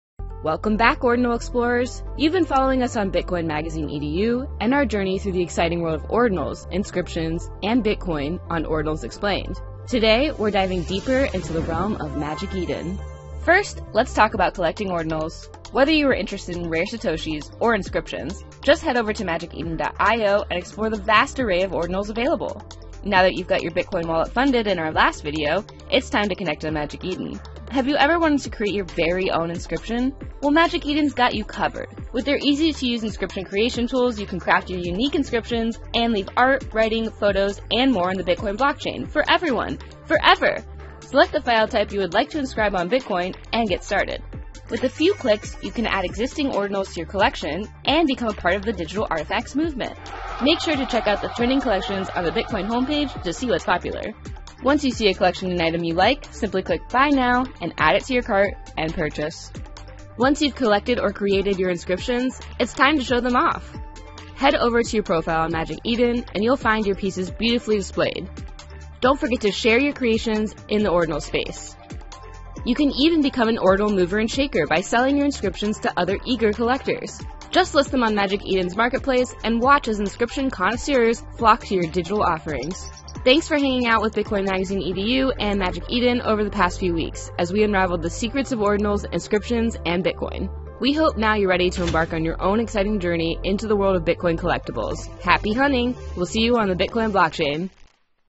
Create and List Your Own Bitcoin Ordinals | Ordinals Explained: Episode 5
Inscriptions on Bitcoin are FOREVER 🤯

Learn how to inscribe, collect & list your very own in the FINALE of Ordinals Explained presented by Magic Eden and Bitcoin Magazine EDU.